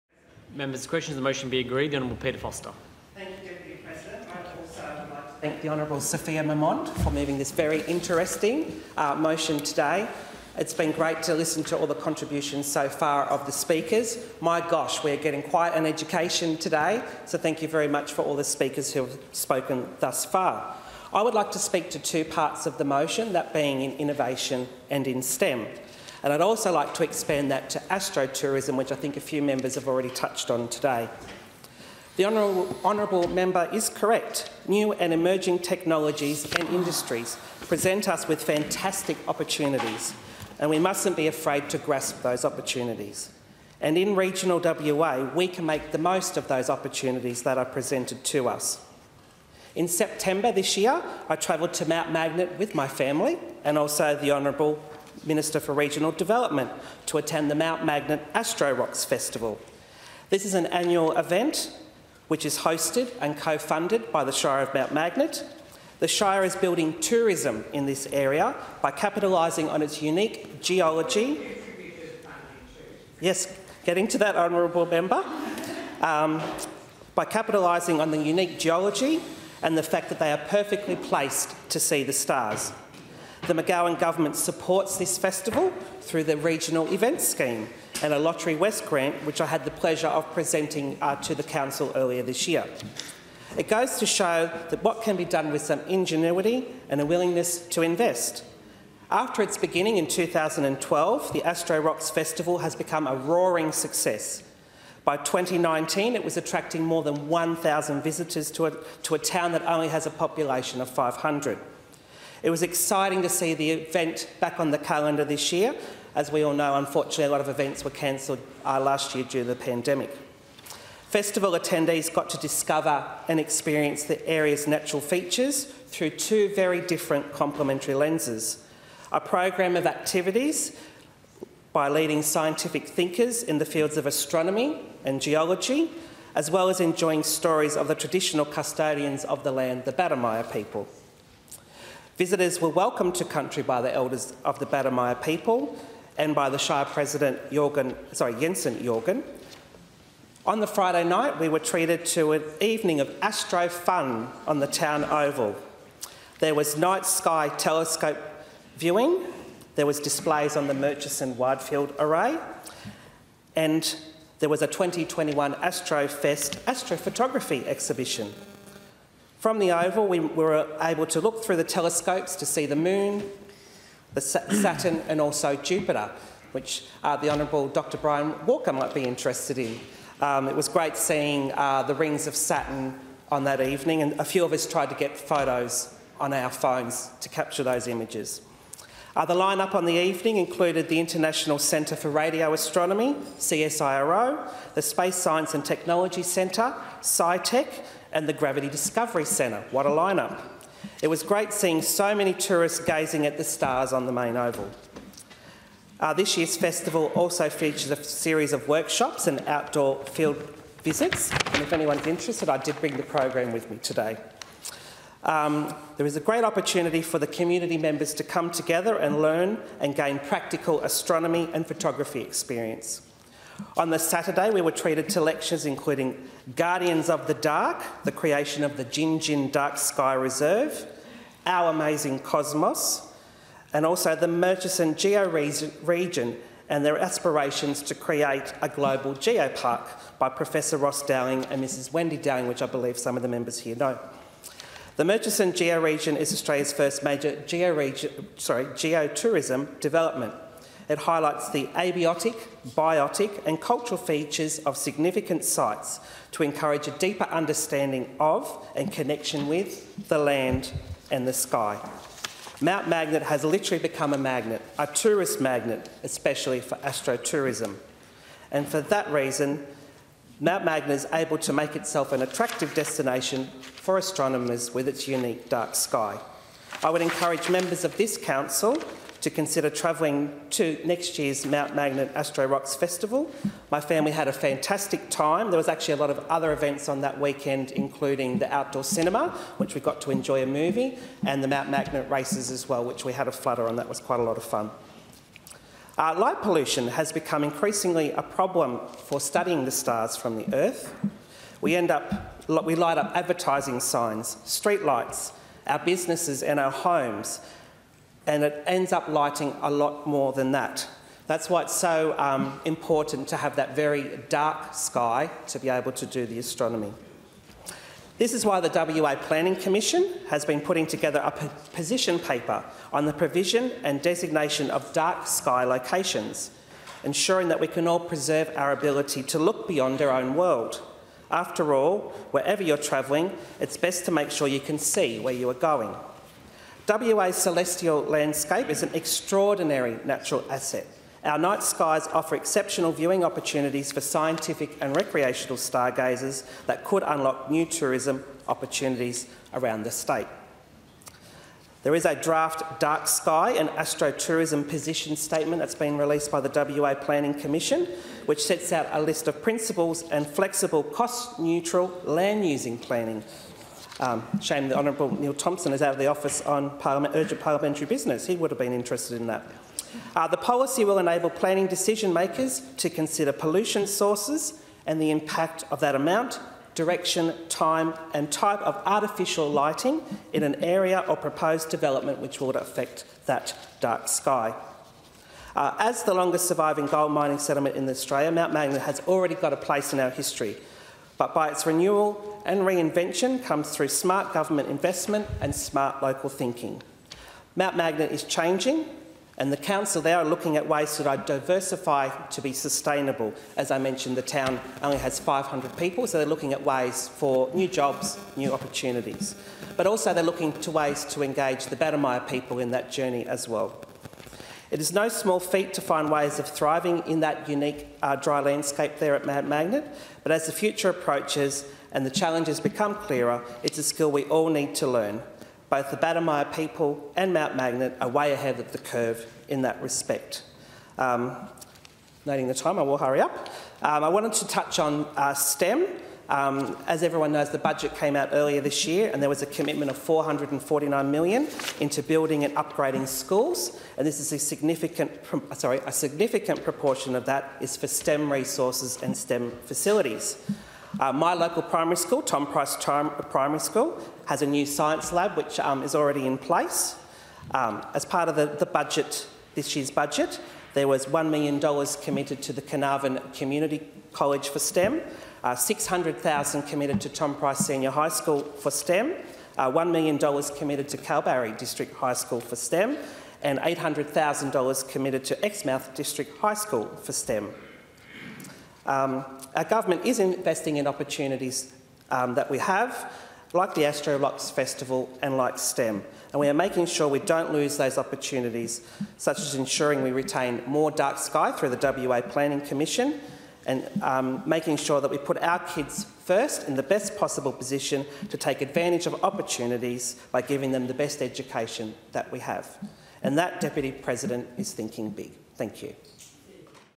Let's Think Big!
Debating a motion from my colleague Sophia Moermond on thinking big in science, tourism, innovation, and the emerging space sector. People in regional WA have a knack for turning their hand to new ideas and taking on new challenges; with the investments we're making in STEM education resources, we're really well placed to take advantage of new technologies and innovations.